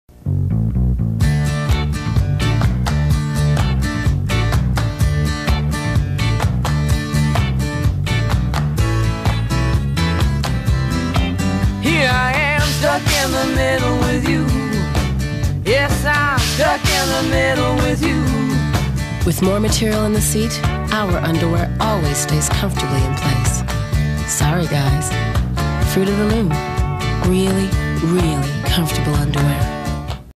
Ron Baxter Smith - Director - Fruit of the Loom - "Stuck"
Director/Cameraman: Ron Baxter Smith
Production Company: Blink Pictures
Executive Producer: Ron Grittani, Jane Kessler
Client: Fruit of the Loom
Agency: Leo Burnett Toronto
Art Director: Elspeth Lynn
Copywriter: Lorraine Tao